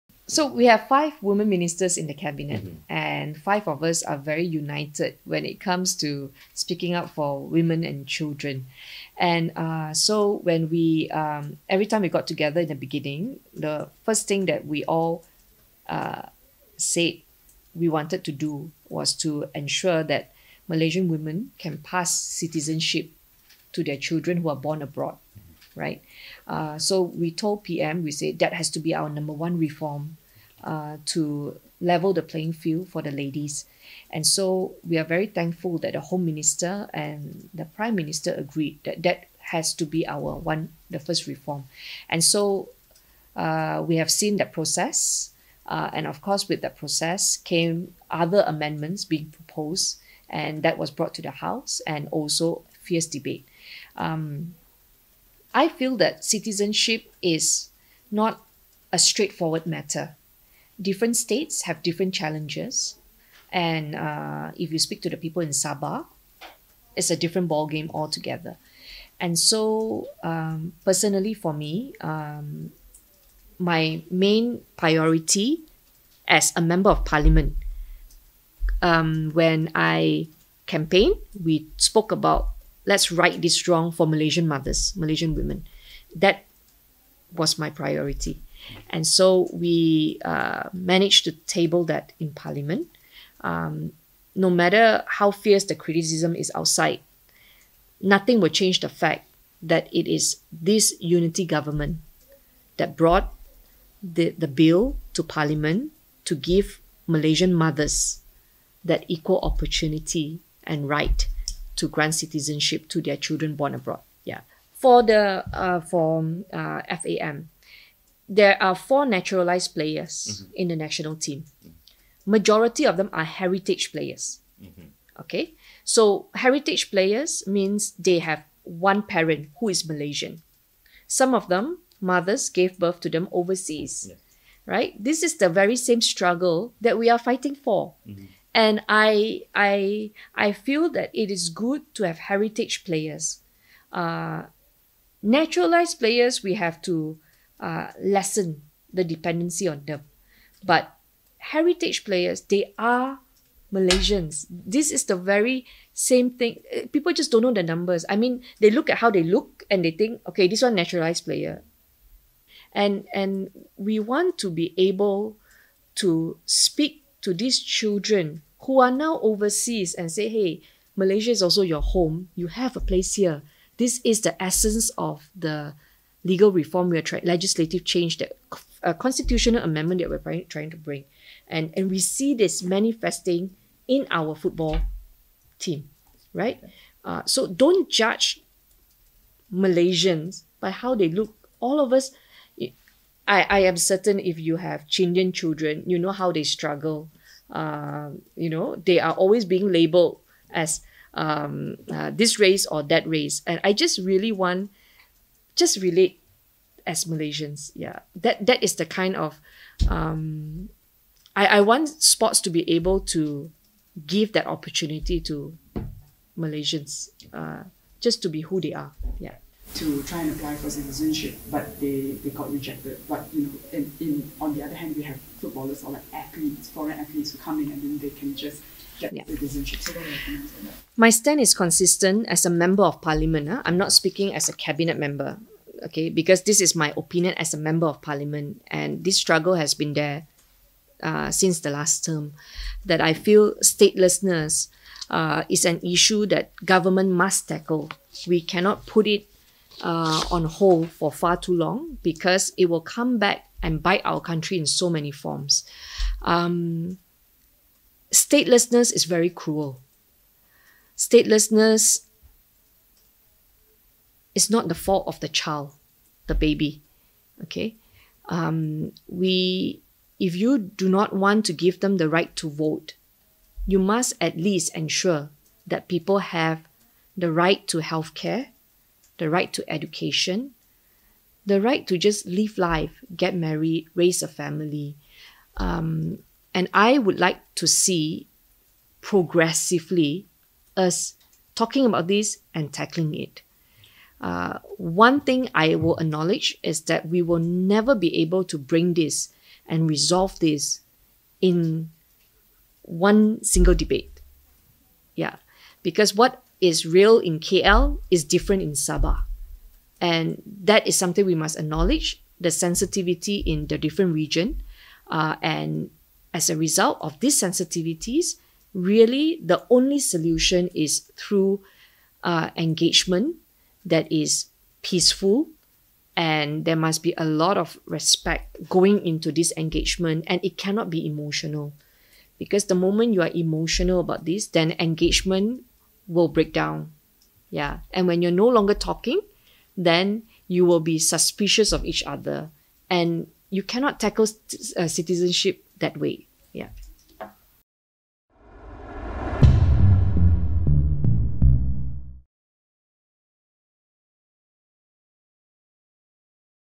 Hannah Yeoh: We tabled citizenship bill to give M’sian mothers equal rights
[Exclusive interview] While acknowledging brickbats surrounding the proposed citizenship law amendments, Youth and Sports Minister Hannah Yeoh was adamant that the part of it which gives equal rights to Malaysian mothers regarding overseas-born children was a positive move.

“Nothing can change the fact that it was the coalition government that tabled the bill in the Parliament to give Malaysian mothers the equal opportunity and right to confer citizenship on their overseas-born children.

“We managed to table the bill. So, for that, I’m very pleased that there was political will to bring it (to the Parliament),” she told Malaysiakini in an exclusive interview at her ministry last Thursday.